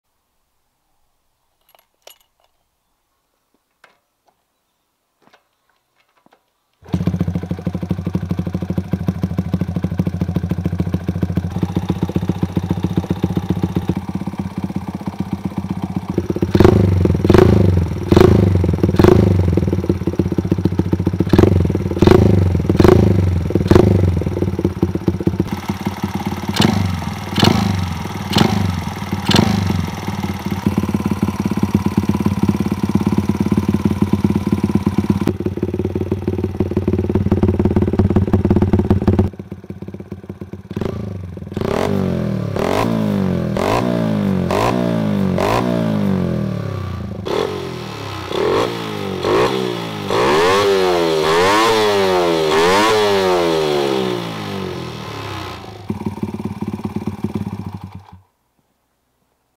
HONDA GORILLA ゴリラ レストア完成後にエンジン始動
レストア後のゴリラ
タケガワ88cc
OVERマフラー
ミクニVM26キャブレター#shorts